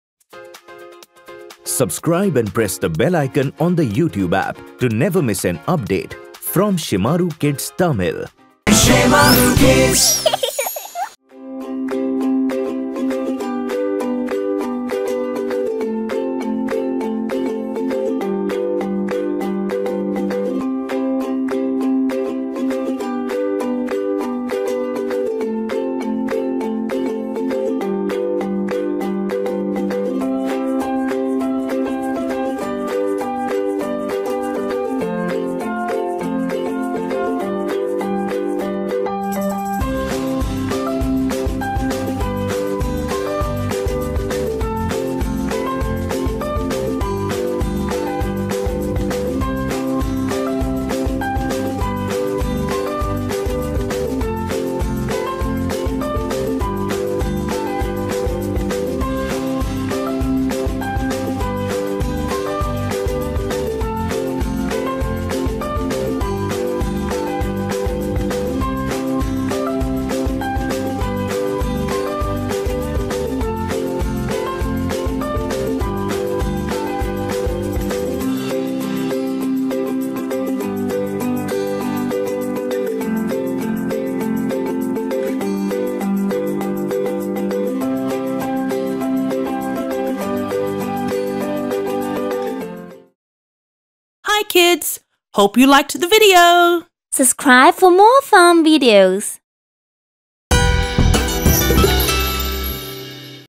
Navratri Special : How To Draw Maa Devi Durga | Shemaroo Kids Tamil
Shemaroo Kids Tamil presents to you an easy and creative way to make a sketch of Goddess Durga. Durga Puja or Sharadotsav is an annual Hindu festival in South Asia that celebrates worship of the Hindu goddessDurga. It refers to all the six days observed as Mahalaya, Shashthi, Maha Saptami, Maha Ashtami, Maha Navami, and Vijayadashami.